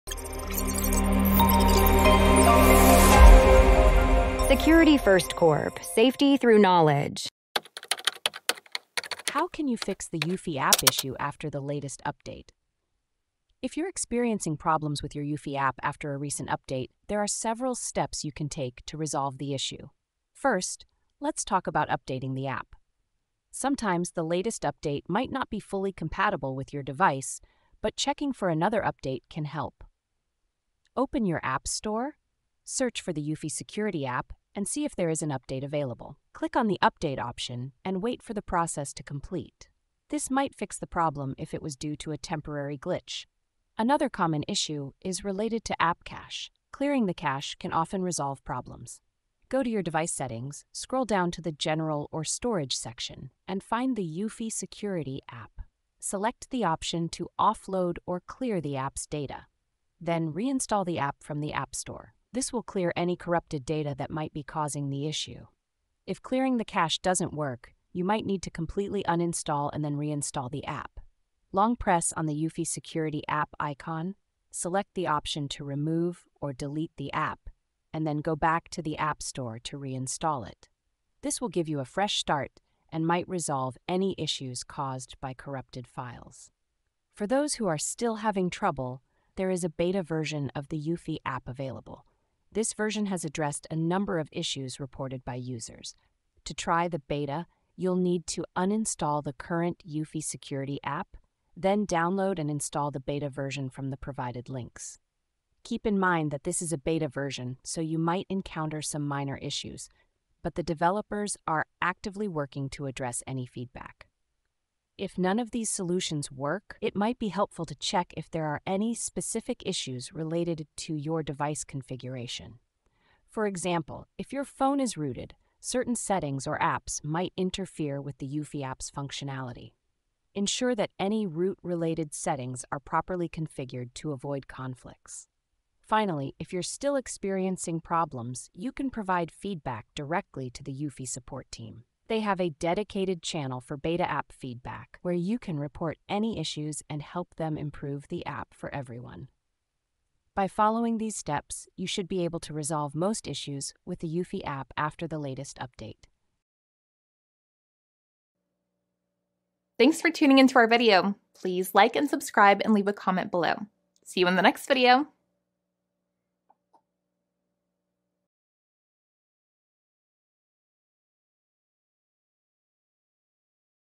How Can You Fix the Eufy App Issue After the Latest Update? | SecurityFirstCorp News
How Can You Fix the Eufy App Issue After the Latest Update? In this video, we will cover the steps you can take to resolve issues with the Eufy Security app following a recent update. Many users have reported problems after updates, and we aim to provide you with practical solutions to get your app functioning smoothly again. We will discuss how to check for updates in your app store, as sometimes a subsequent update can resolve any compatibility issues. Additionally, we will guide you on clearing the app cache and data, which may help eliminate any corrupted files causing the app to misbehave.

 If those steps do not work, we will show you how to uninstall and reinstall the app for a fresh start. For users who want to try something different, we will introduce the beta version of the Eufy app, which may fix some of the issues you are encountering. Lastly, we will provide tips on addressing device-specific concerns, especially for those with rooted devices, and how to reach out to Eufy support for further assistance.